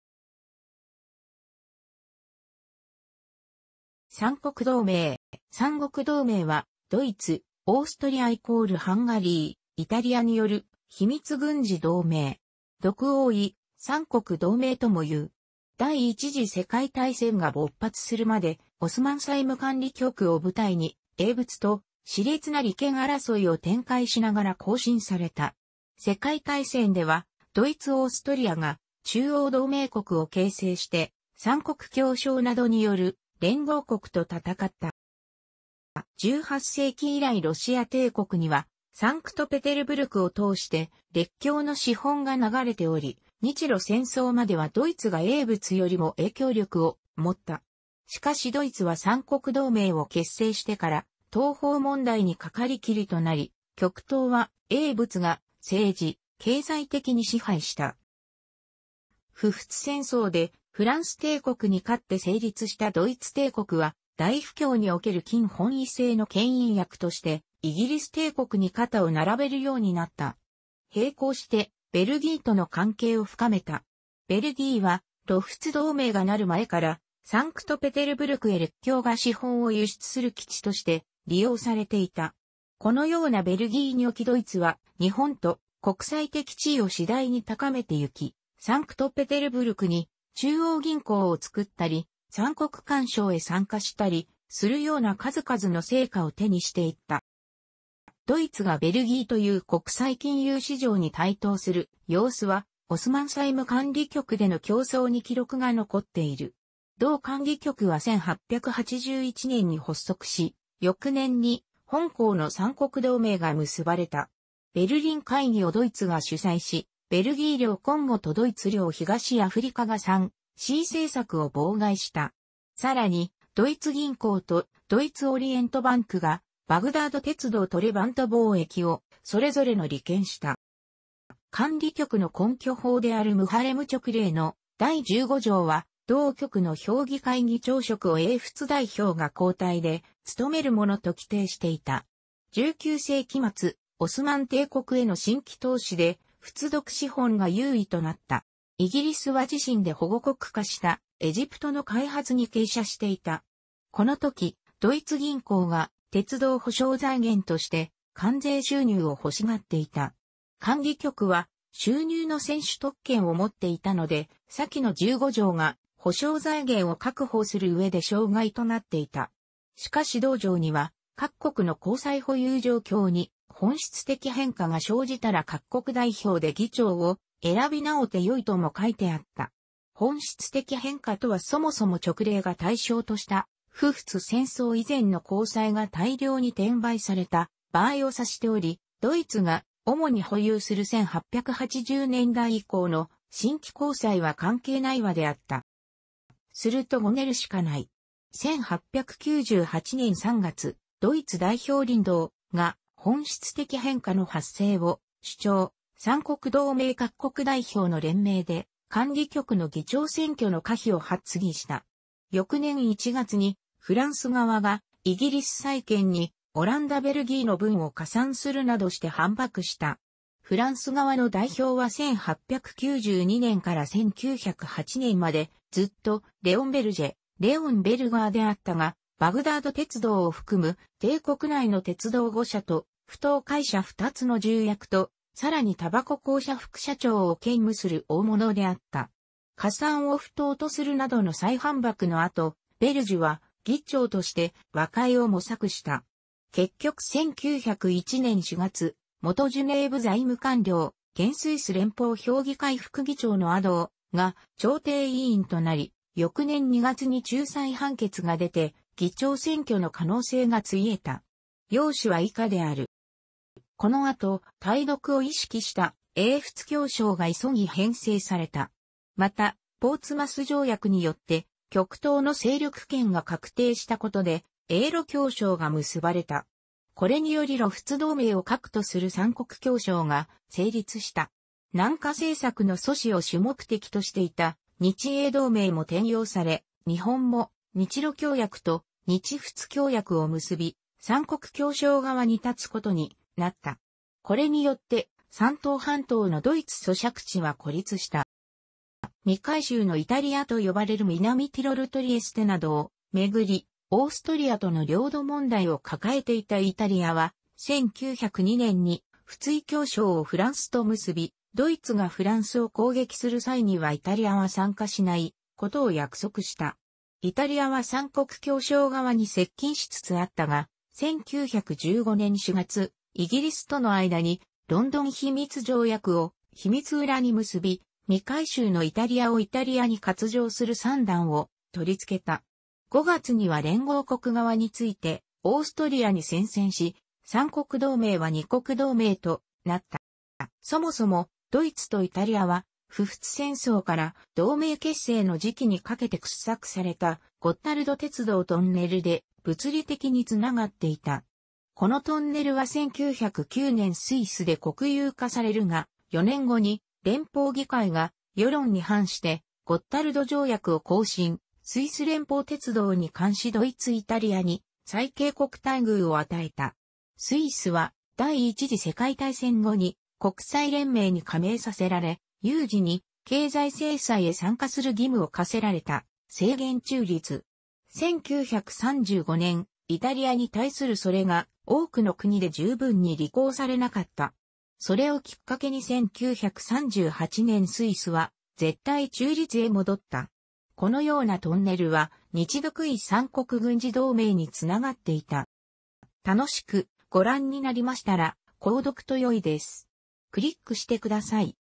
三国同盟 (1882年)
オーストリア＝ハンガリー帝国の国際関係

三国同盟（さんごくどうめい）は、ドイツ、オーストリア＝ハンガリー、イタリアによる秘密軍事同盟。独墺伊（三国）同盟ともいう。第一次世界大戦が勃発するまで、オスマン債務管理局を舞台に英仏と熾烈な利権争いを展開しながら更新された。世界大戦ではドイツ・オーストリアが中央同盟国を形成して、三国協商などによる連合国と戦った。

18世紀以来ロシア帝国にはサンクトペテルブルクを通して列強の資本が流れており、日露戦争まではドイツが英仏よりも影響力をもった。しかしドイツは三国同盟を結成してから東方問題にかかりきりとなり、極東は英仏が政治・経済的に支配した。

普仏戦争でフランス帝国に勝って成立したドイツ帝国は、大不況における金本位制の牽引役としてイギリス帝国に肩を並べるようになった。並行してベルギーとの関係を深めた。ベルギーは露仏同盟が成る前から、サンクトペテルブルクへ列強が資本を輸出する基地として利用されていた。このようなベルギーにおきドイツは日本と国際的地位を次第に高めてゆき、サンクトペテルブルクに中央銀行をつくったり、三国干渉へ参加したりするような数々の成果を手にしていった。

ドイツがベルギーという国際金融市場に台頭する様子は、オスマン債務管理局での競争に記録が残っている。同管理局は1881年に発足し、翌年に本稿の三国同盟が結ばれた。ベルリン会議をドイツが主催し、ベルギー領コンゴとドイツ領東アフリカが3C政策を妨害した。さらにドイツ銀行とドイツ・オリエントバンクがバグダード鉄道とレバント貿易をそれぞれの利権とした。

管理局の根拠法であるムハレム勅令の第15条は、同局の評議会議長職を英仏代表が交代で務めるものと規定していた。19世紀末、オスマン帝国への新規投資で仏独資本が優位となった。イギリスは自身で保護国化したエジプトの開発に傾斜していた。このとき、ドイツ銀行が鉄道保証財源として関税収入をほしがっていた。管理局は収入の先取特権をもっていたので、先の15条が保証財源を確保する上で障害となっていた。しかし同条には、各国の公債保有状況に「本質的変化」が生じたら各国代表で議長を選びなおしてよいとも書いてあった。「本質的変化」とはそもそも勅令が対象とした普仏戦争以前の公債が大量に転売された場合をさしており、ドイツが主に保有する1880年代以降の新規公債は関係ないはずであった。

するとごねるしかない。1898年3月、ドイツ代表リンドウ（）が「本質的変化」の発生を主張、三国同盟各国代表の連名で管理局の議長選挙の可否を発議した。翌年1月にフランス側がイギリス債権にオランダ・ベルギーの分を加算するなどして反駁した。フランス側の代表は1892年から1908年までずっとレオン・ベルジェ（Léon Berger）であったが、バグダード鉄道をふくむ帝国内の鉄道5社と埠頭会社2つの重役と、さらにたばこ公社副社長を兼務する大物であった。加算を不当とするなどの再反駁の後、ベルジュは議長として和解を模索した。結局1901年4月、元ジュネーブ財務官僚・現スイス連邦評議会副議長のアドー（）が調停委員となり、翌年2月に仲裁判決が出て議長選挙の可能性が潰えた。要旨は以下である。

このあと、対独を意識した英仏協商が急ぎ編成された。また、ポーツマス条約によって極東の勢力圏が確定したことで、英露協商が結ばれた。これにより露仏同盟を核とする三国協商が成立した。南下政策の阻止を主目的としていた日英同盟も転用され、日本も日露協約と日仏協約を結び、三国協商側に立つことになった。これによって山東半島のドイツ租借地は孤立した。

「未回収のイタリア」と呼ばれる南ティロル・トリエステなどを巡りオーストリアとの領土問題を抱えていたイタリアは、1902年に仏伊協商をフランスと結び、ドイツがフランスを攻撃する際にはイタリアは参加しないことを約束した。イタリアは三国協商側に接近しつつあったが、1915年4月イギリスとの間にロンドン秘密条約を秘密裏に結び、未回収のイタリアをイタリアに割譲する算段をとりつけた。5月には連合国側に付いてオーストリアに宣戦し、三国同盟は二国同盟となった。

そもそもドイツとイタリアは、普仏戦争から同盟結成の時期にかけて掘削されたゴッタルド鉄道トンネルで物理的につながっていた。このトン...